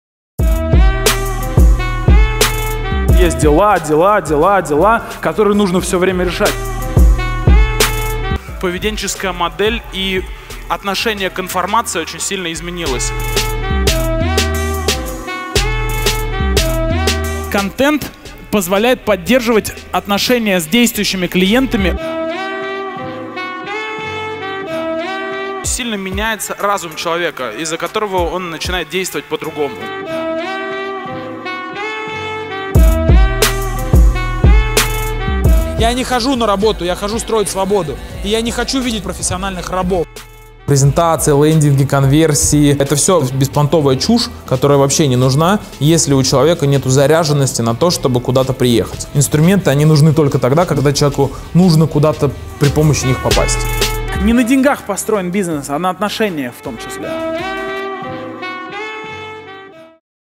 Илья Кусакин в Кишиневе
Чтобы стать успешным в продажах, мало выучить скрипты и взять в руки телефон. Нужно знать и понимать все о бизнесе и продажах системно и досконально одновременно.

Именно такой подход использует Илья Кусакин: все зарубежные методики он адаптирует для СНГ, превращая в понятные и работающие инструменты. Взял - внедрил - получил “отдачу”.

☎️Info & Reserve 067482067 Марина